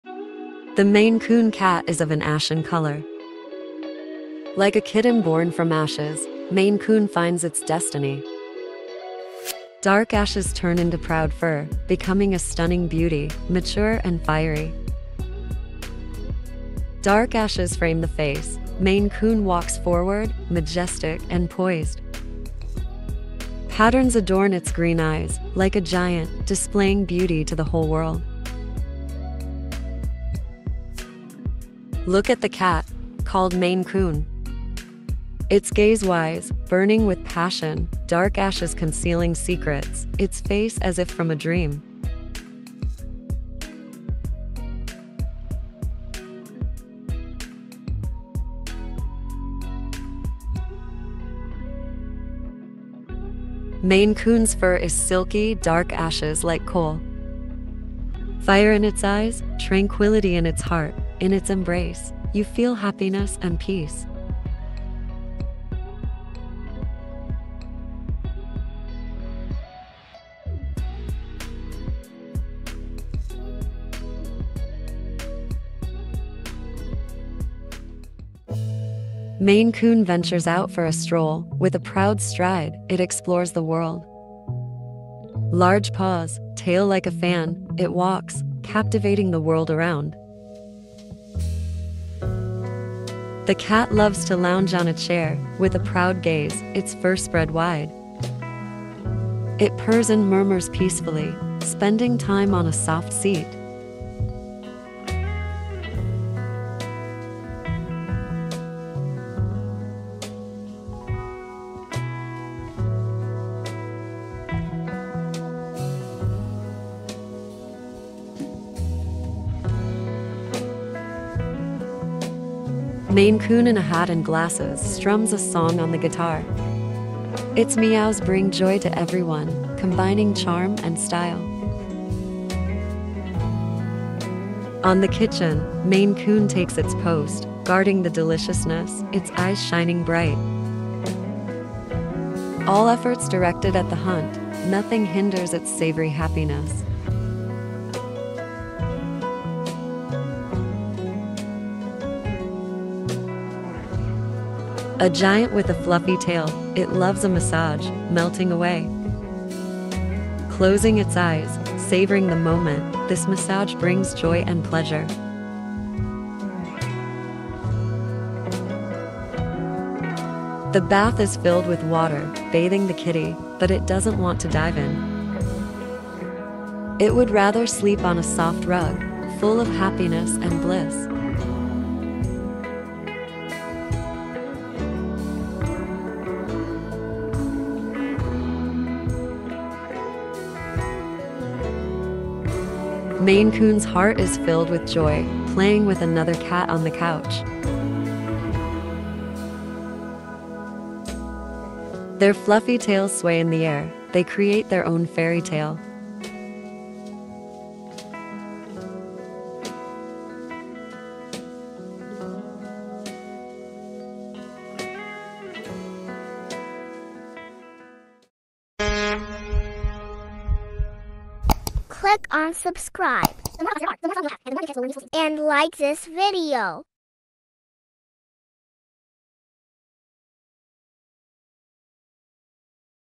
Maine Coon: Story for Children about Beautiful and Big Maine Coon Cat | Sing along in English
Watch a story for children about a beautiful and big, black Maine Coon cat on the "Sing Along in English" channel. Explore cats, their personality, fur patterns, traits, and individuality. These charming feline companions are sure to delight animal lovers of all ages. Don't miss out on the fun adventure with these adorable creatures on the "Sing Along in English" channel.

◢ Tracklist:
00:01 From Kitten to Adult Cat
00:17 Examining the Details of the Cat
01:19 Portrait
01:33 Feather
02:03 Hat
02:14 Kitchen
02:36 Massage
02:50 Bathing
03:13 Playing with Another Cat
03:39 Subscribe and go to other videos of the channel